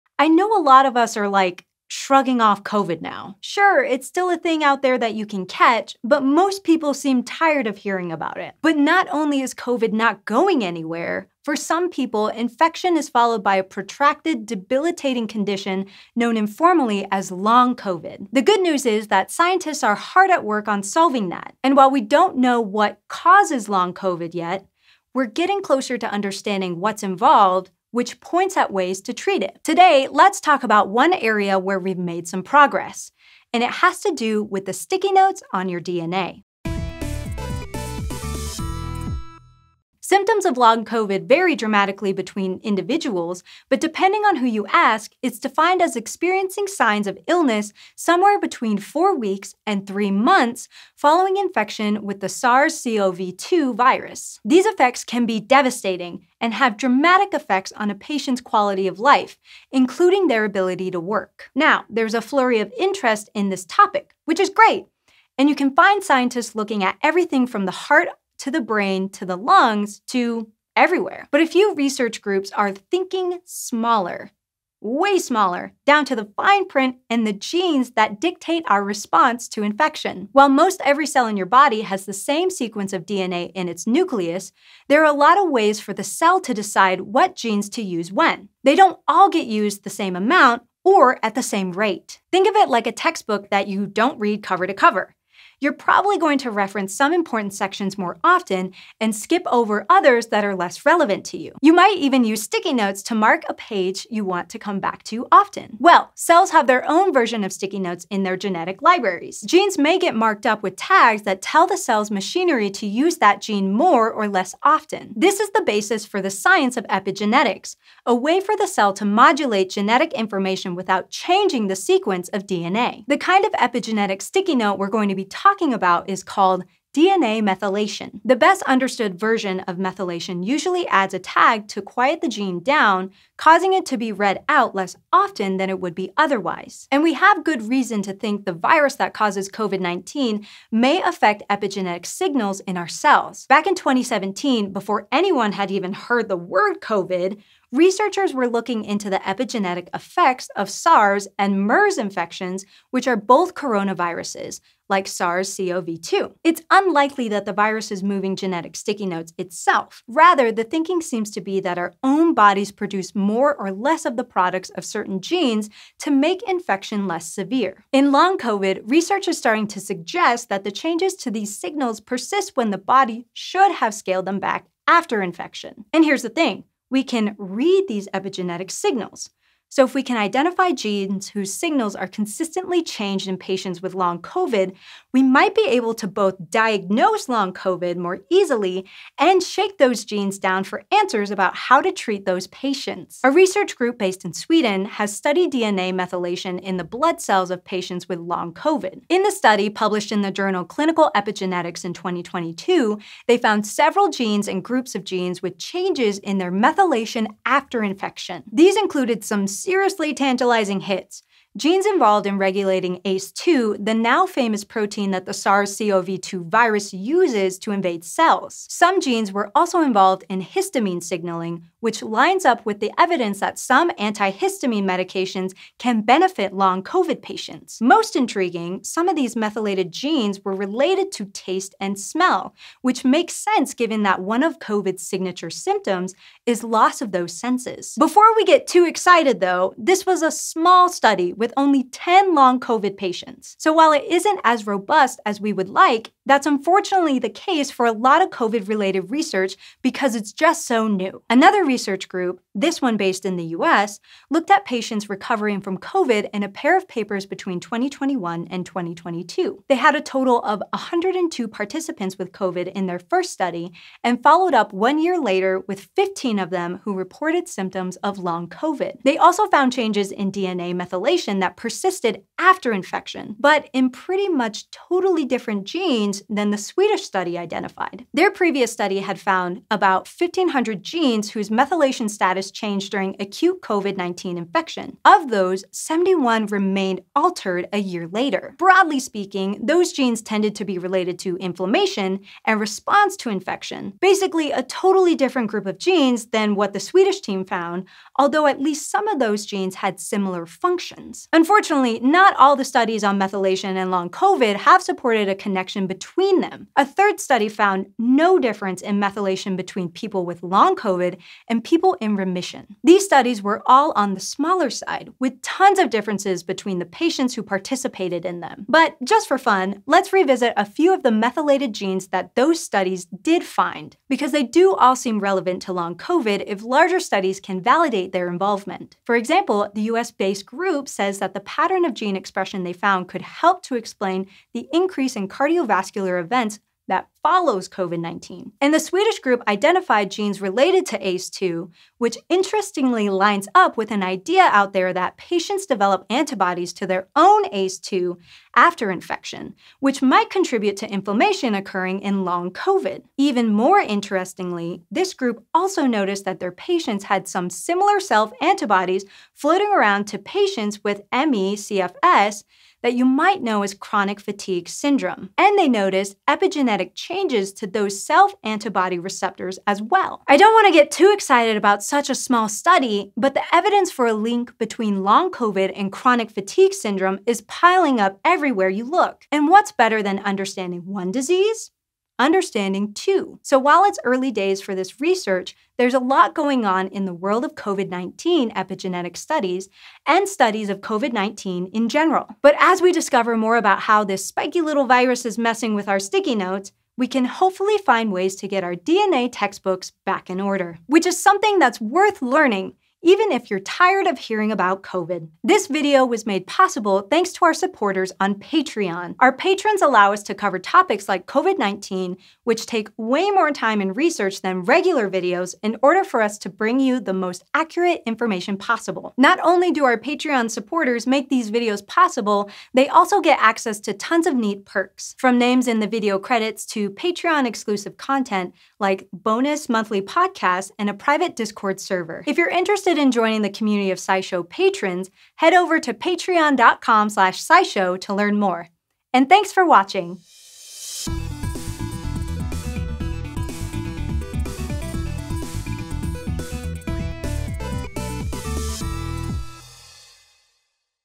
Does COVID Mess With Your DNA?
As more and more are infected with COVID-19, there's a growing group of people who have what's called Long COVID, meaning they still have symptoms for weeks or months after getting sick. While we still don't know for sure the cause of long COVID, there's a growing body of research that long COVID may be the result of the virus messing with people's DNA.

Hosted by: Rose Bear Don't Walk

Sources:

Sources:
Swedish studies:

Images